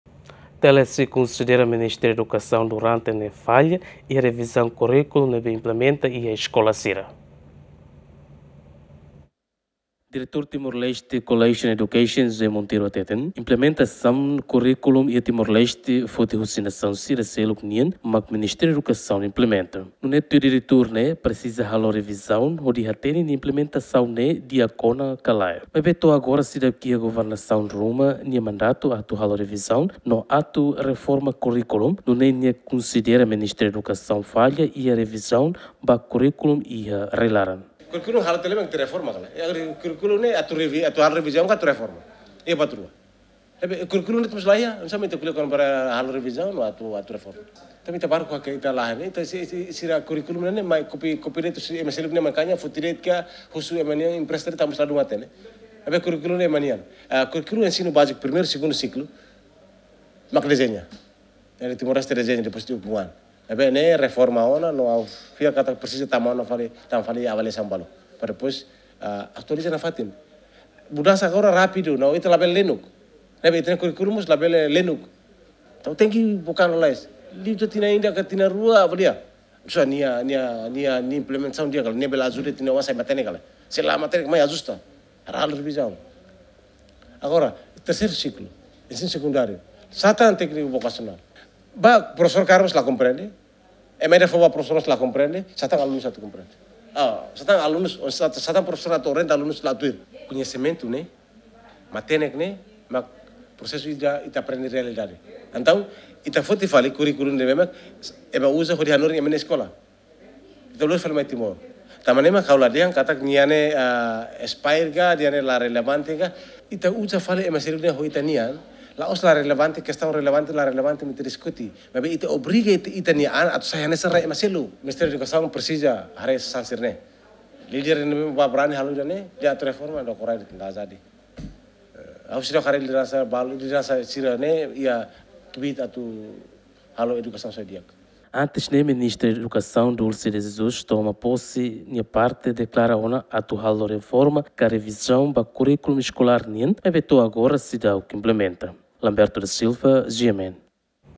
TLCE konsidera ME durante ne’e faila iha revisaun kurikulum ne’ebé mak implenta iha eskola sira
TLCE konsidera Ministériu Edukasaun durante ne’e faila iha revisaun kurikulum ne’ebé mak implenta iha eskola sira